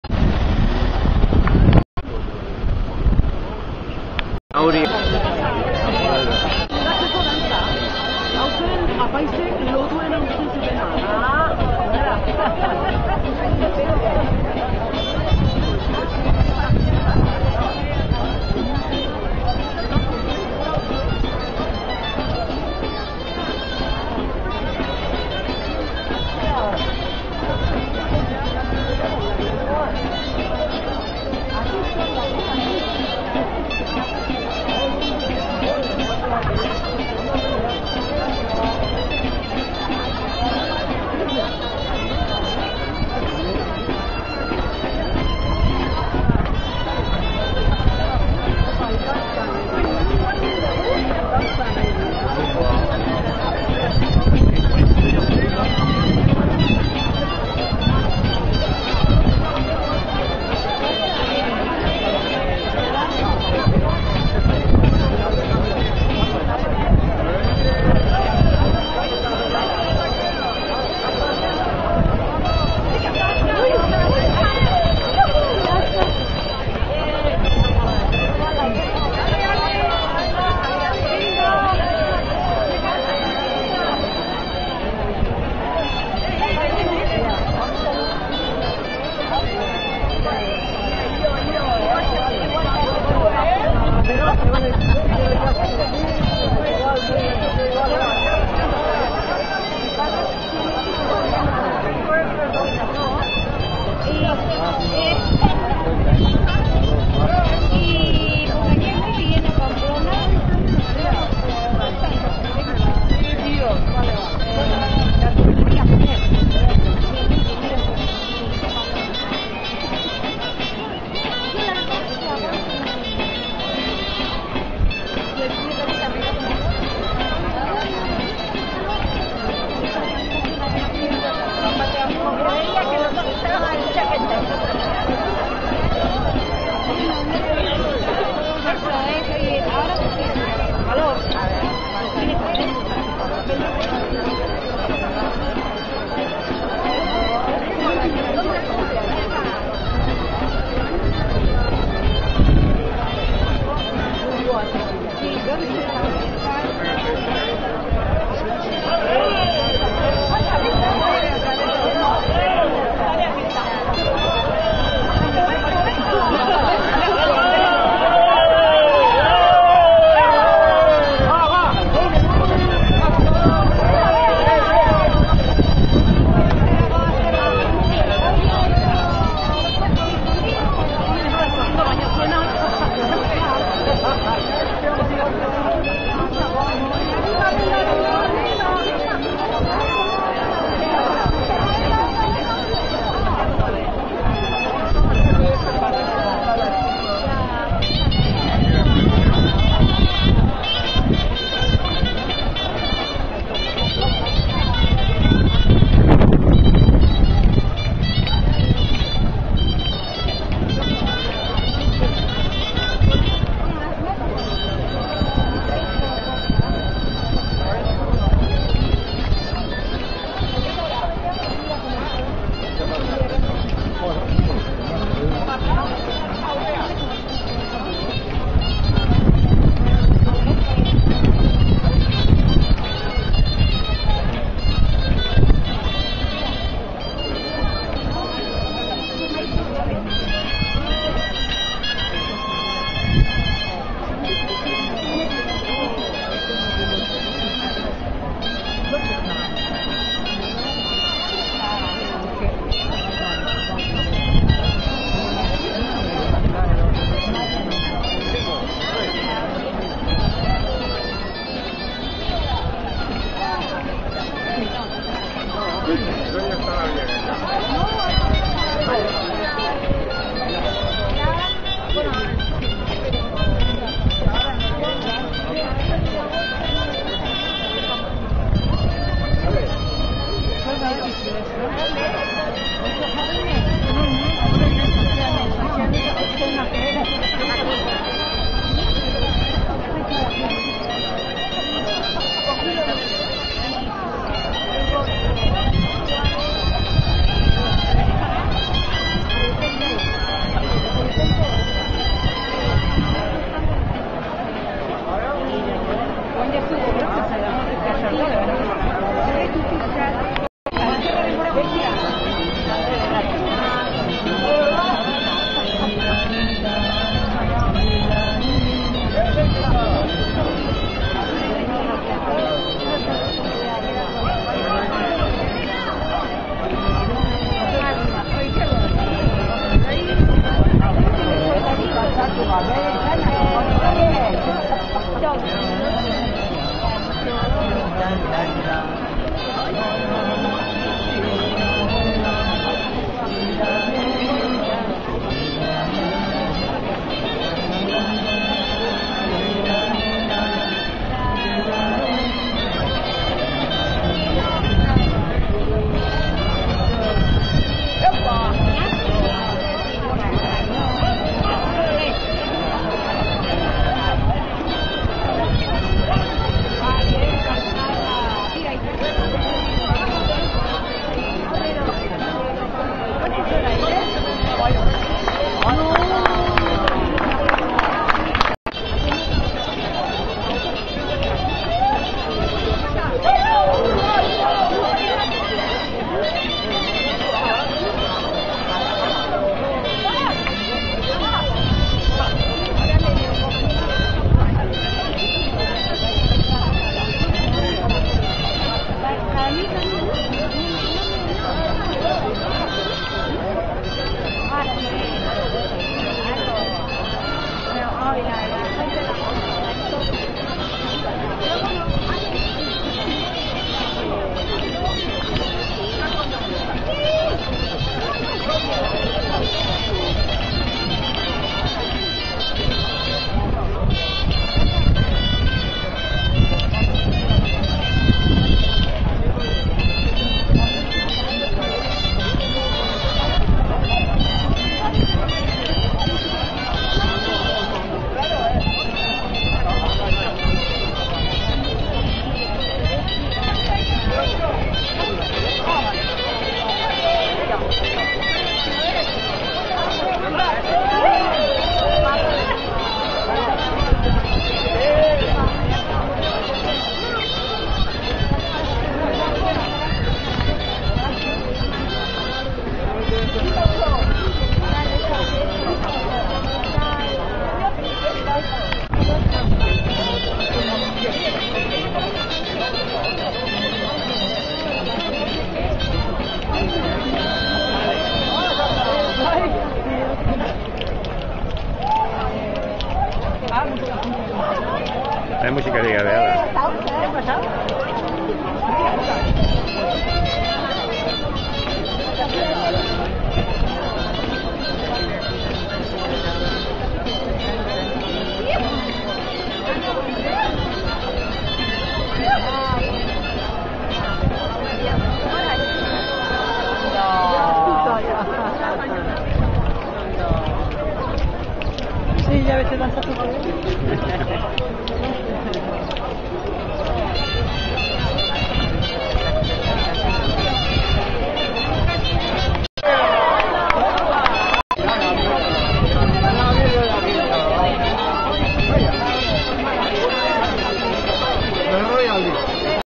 Larrain dantza Karmen jaiak Donostiak 2019-7-14
Larrain dantza erromeria amaitzeko Karmengo jaietan Donostiako kaian.